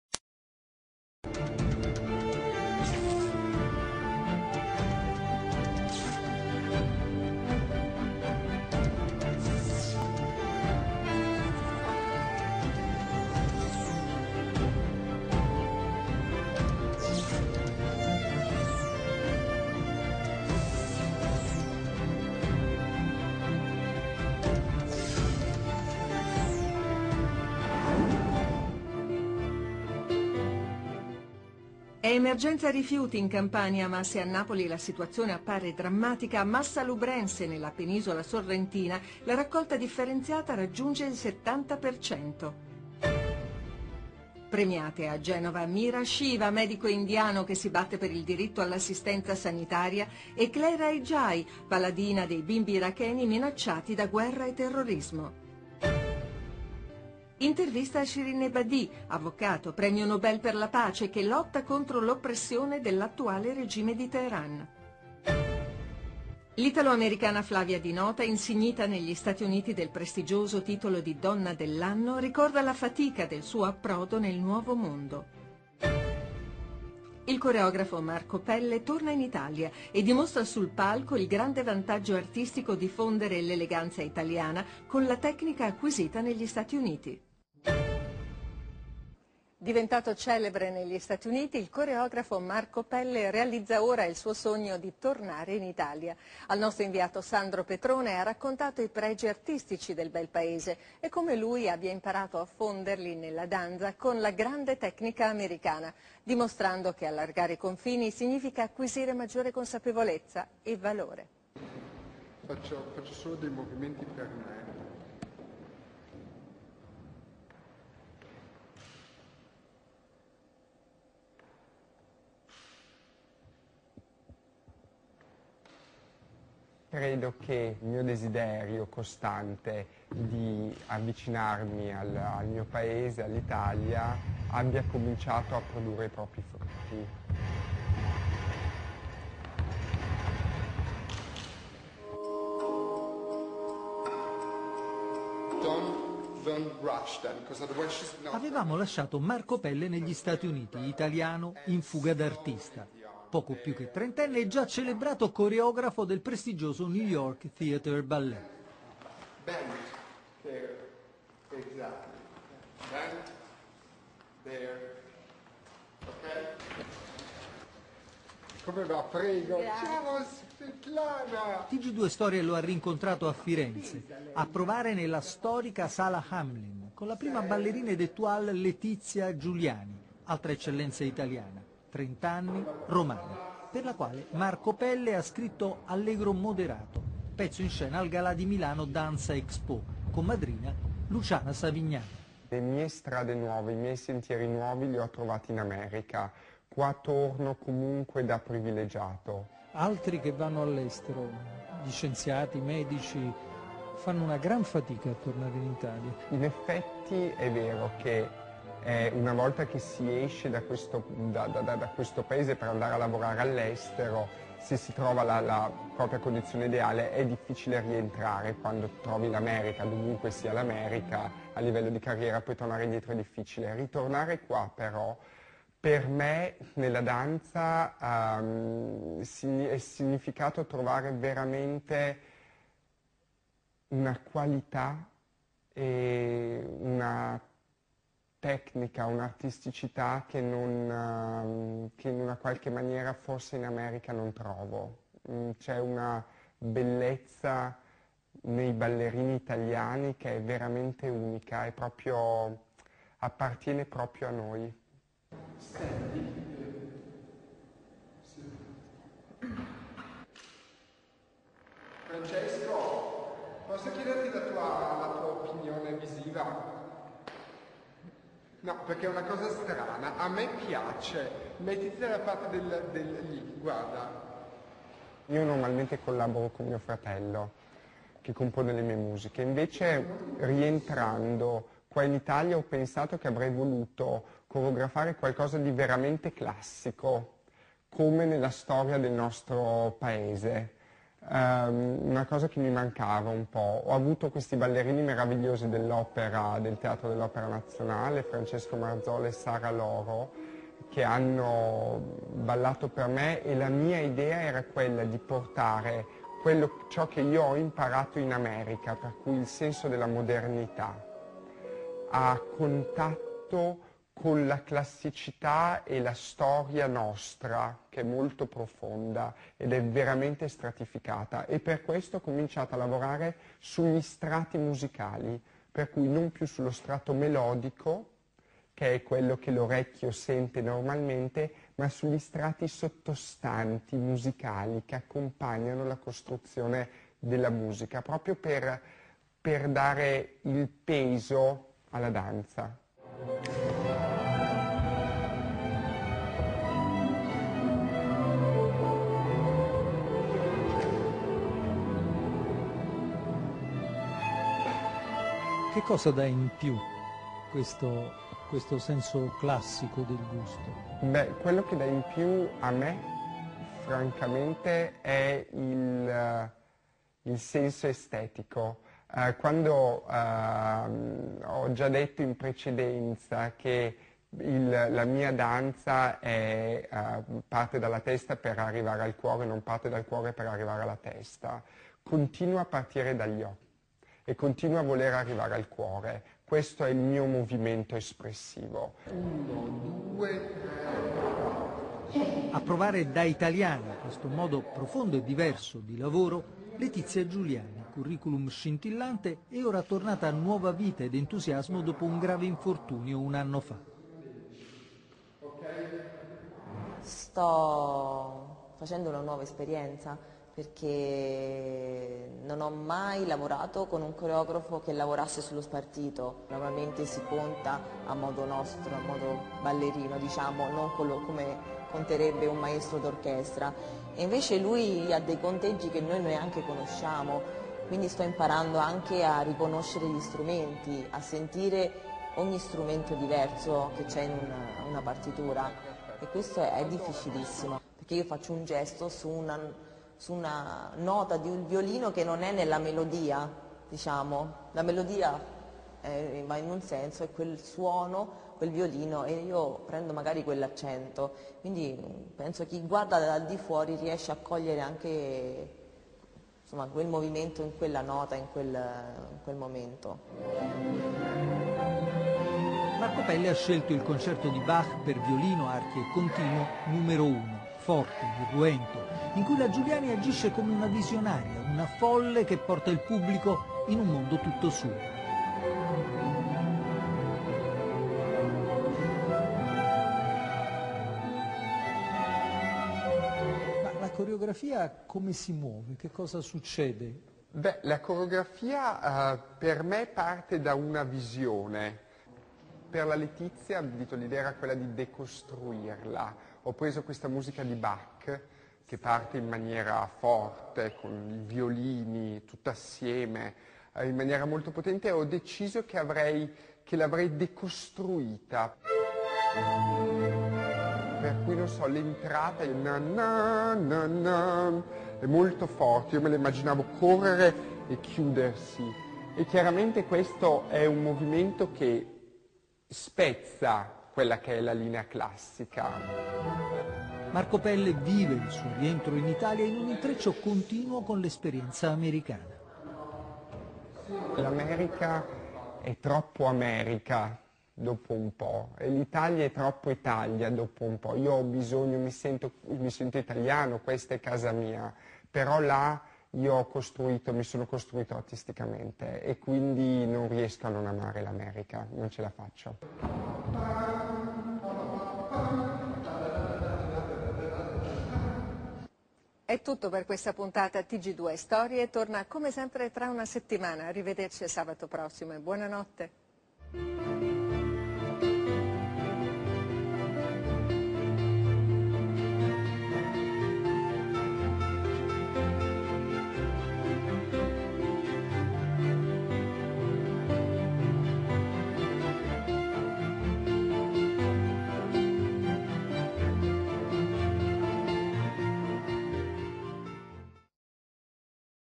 Intervista a Marco Pelle per TG2 Storie
Sandro Petrone intervista Marco Pelle per TG2 Storie riguardo il nuovo lavoro creato apposta per la Prima Ballerina Lutizia Giuliani. Cameo di Francesco Marzola.